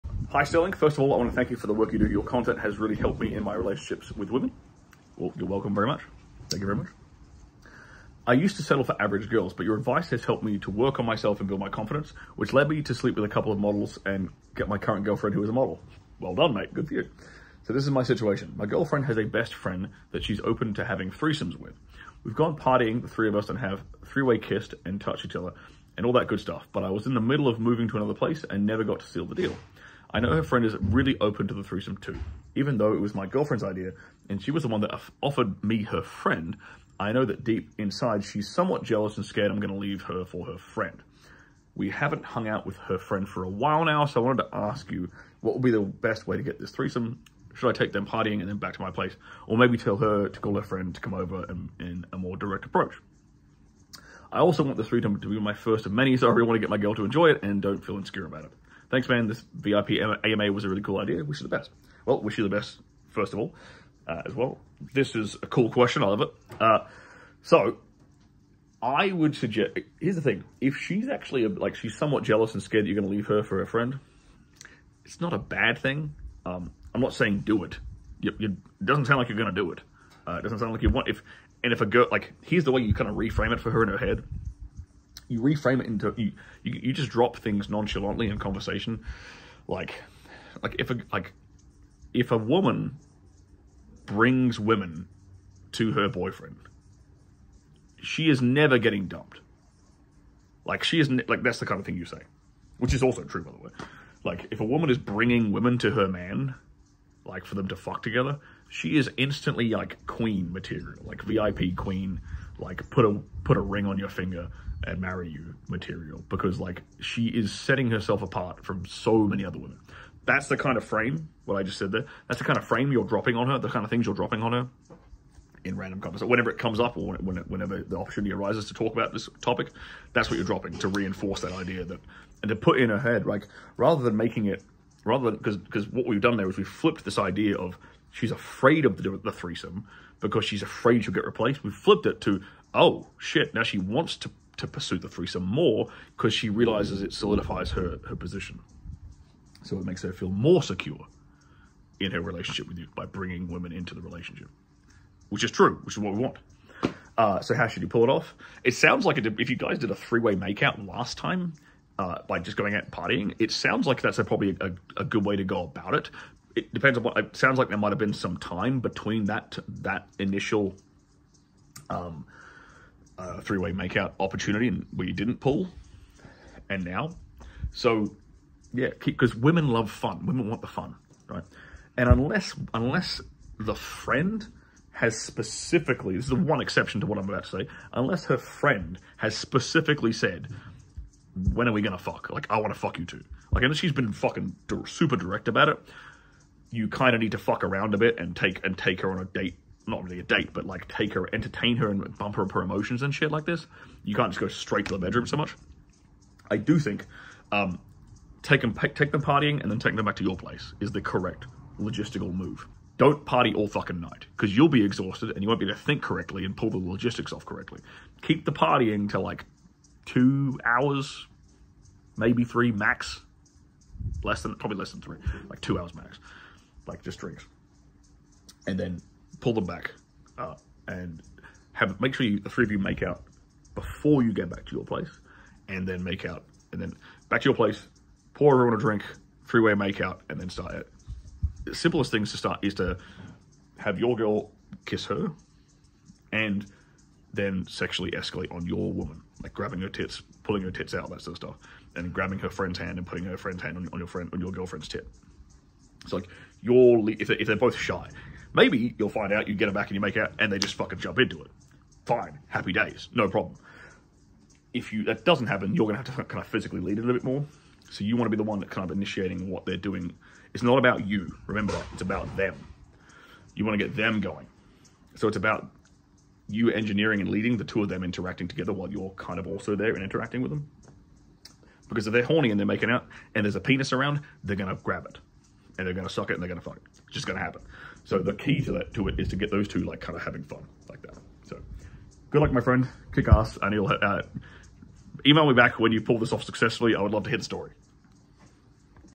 Initiate a Threesome with Girlfriend's Friend in 3 steps
Threesome with girlfriend's friend. A correspondent thanks sex coach Stirling Cooper for his advice which helped the correspondent improve their confidence and attract a model girlfriend.

He seeks advice on engaging in a threesome with his girlfriend and her best friend, expressing concerns about potential jealousy. Stirling advises reframing the situation to make the girlfriend feel more secure and suggests subtly reinforcing the idea that bringing other women to a man strengthens the relationship. He recommends slowly escalating physical intimacy during a night out with the two women before transitioning to a three-way encounter at the correspondents's place. Stirling emphasizes that the interaction is not about the man but, instead, about the two women. It is important to focusing on the enjoyment and comfort of both women during the encounter. The man should lead and provide guidance to initiate their sexual interaction.

In this AMA the correspondent is Lionel F. from the USA.

💊 Last Longer - Prevent Premature Ejaculation
💊 Male Enhancement - Rock Hard Bloodflow
💊 Libido Booster - Go Multiple Rounds Recover Fast

Timestamps:

0:00  How to seal the deal with a threesome
0:33  Jealousy in threesome with girlfriend's friend
1:04  Reframe: bringing women to a man is good
2:17  She wont get replaced
2:38  Take the 2 women partying then to your place
3:56  Three-way make-out then lead what they do
4:27  It's not about you, it's about them

CS240627PV1